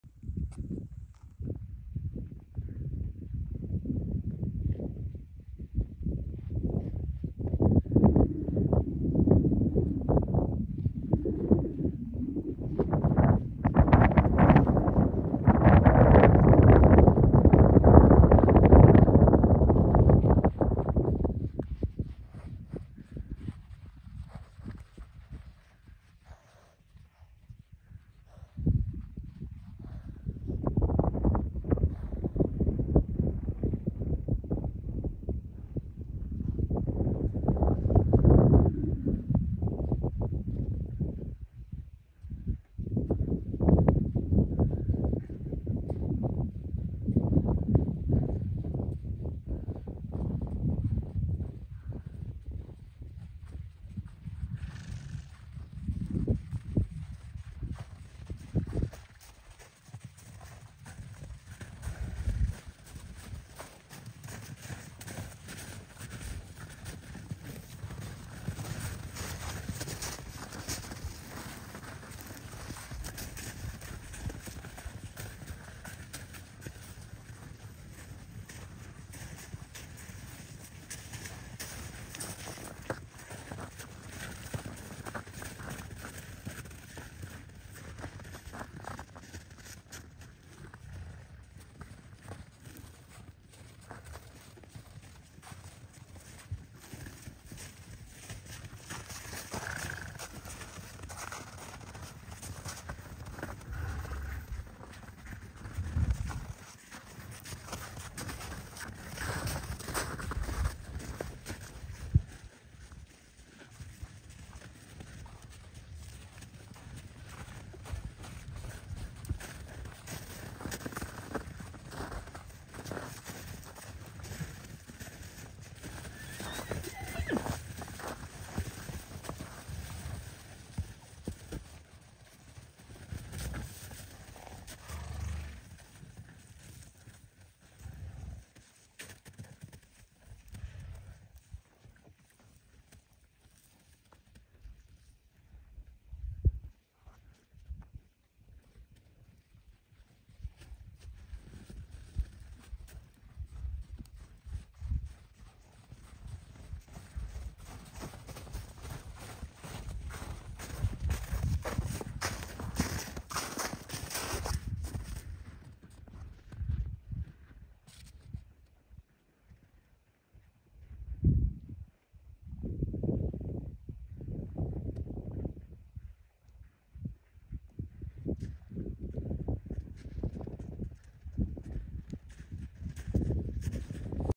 Apollo and the boys
Here, finally is the long version of the beautiful video of Apollo, Aslan and Hermes, running up the hill with the rest of the boys behind them.

I put the longer one up here and filmed it sideways so it’s good to watch on the big screen.  Enjoy the breathtaking sight of a band of rescued wild horses running across a snowy landscape. It took my breath away.